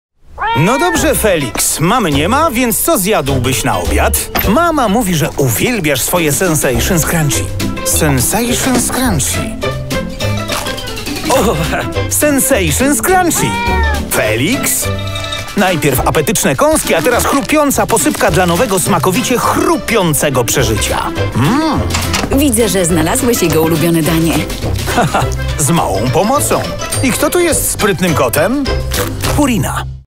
Felix Sensations Crunchy [marketing-news.pl]
Reklama z kampanii nowej kociej karmy marki Felix (Purina).
    Adaptacja spotu na polski rynek: McCann Warsaw
    Zakup mediów: MEC